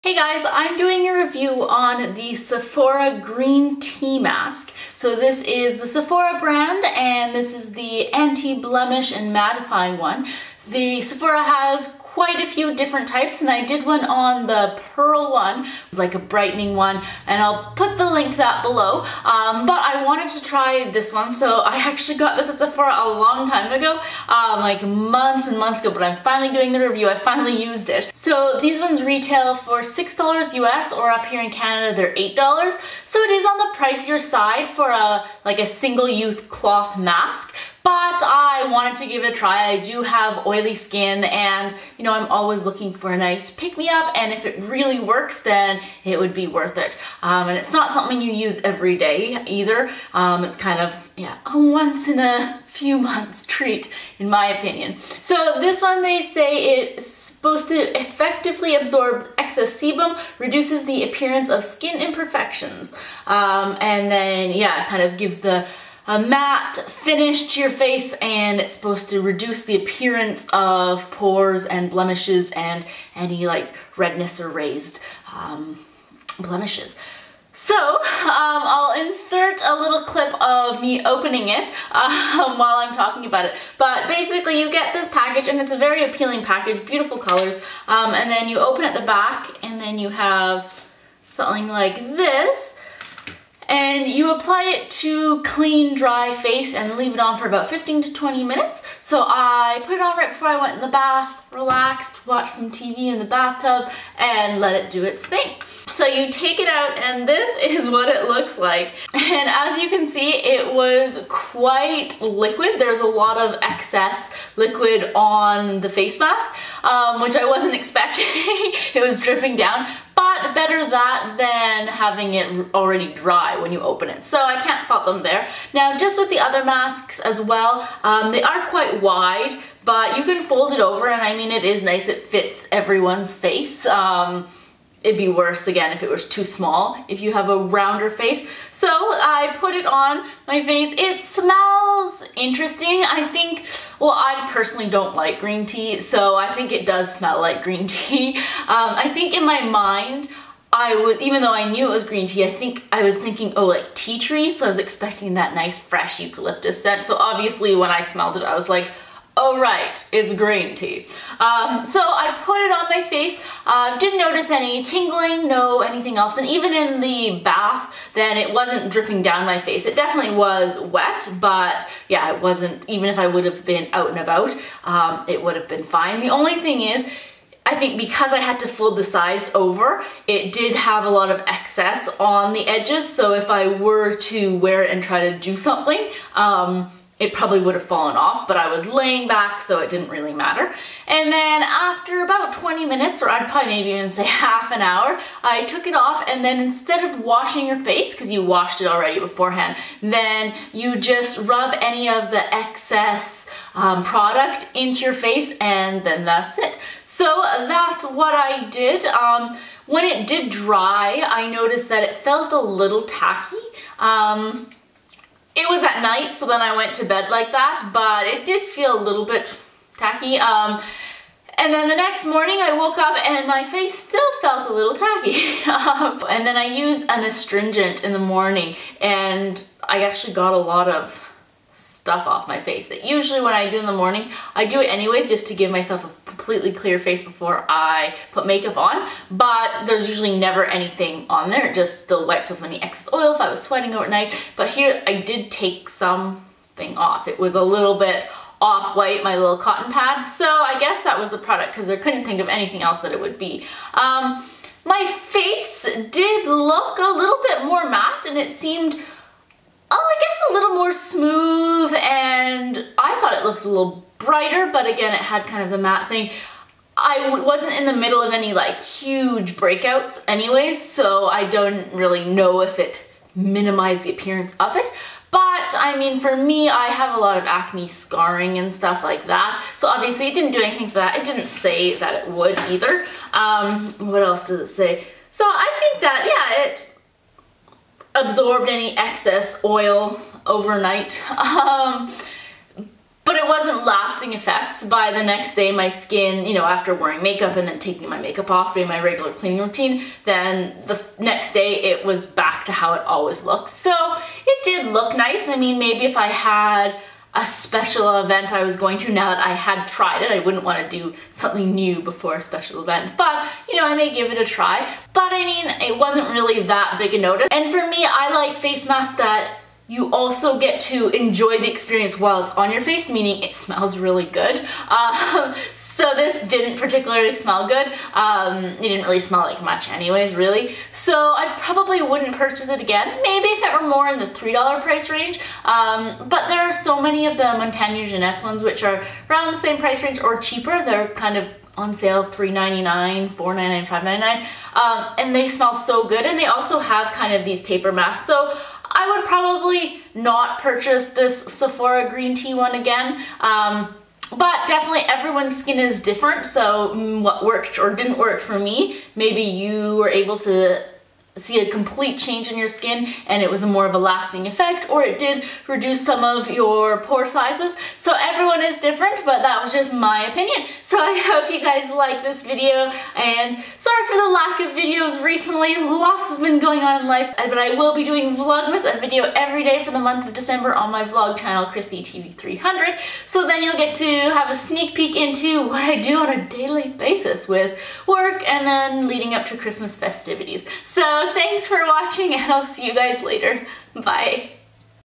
Review: Sephora Green Tea Mask - Mattifying & Anti-Blemish
Sephora's Green Tea Mask on Oily Skin Review

Send me a card or postcard:

Christy Len
Victoria, BC
V8T5G2
Canada